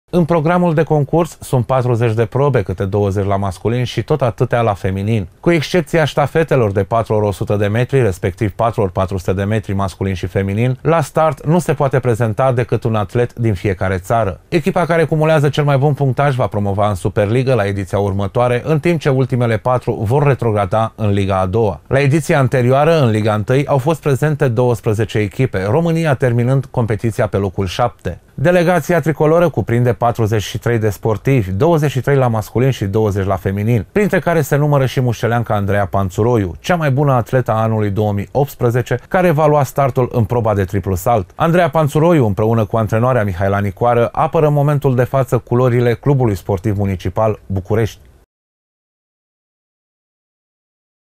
stire 07 08 2019 Andreea Panţuroiu, la europeanul din Norvegia
În perioada 9-11 august, la Sandnes, Norvegia, se va desfăşura cea de-a opta ediţie a Campionatului European pe echipe Liga I, unde România se va lupta pentru accederea în Superligă. Din echipa tricoloră deplasată în Norvegia face parte şi musceleanca, Andreea Panţuroiu, cea mai bună atletă a anului 2018.